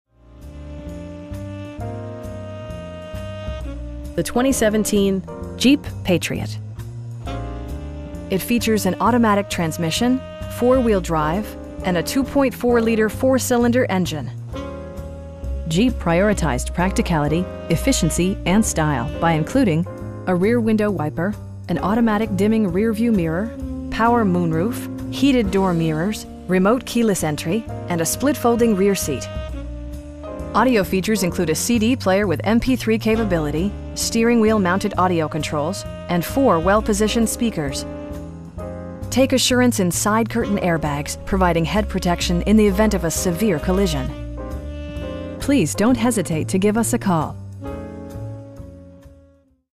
2017 Jeep Patriot Sport 4x4 in Chesapeake, VA 23320
Southern Chrysler Jeep - Greenbrier
1414 South Military Highway

The 2017 Jeep Patriot. It features an automatic transmission, 4-wheel drive, and a 2.4 liter 4 cylinder engine. Jeep prioritized practicality, efficiency, and style by including: a rear window wiper, an automatic dimming rear-view mirror, front bucket seats, power moon roof, heated door mirrors, remote keyless entry, and a split folding rear seat. Audio features include a CD player with MP3 capability, steering wheel mounted audio controls, and 4 speakers, providing excellent sound throughout the cabin. Take assurance in side curtain airbags, providing head protection in the event of a severe collision. We pride ourselves in consistently exceeding our customer's expectations. Please don't hesitate to give us a call.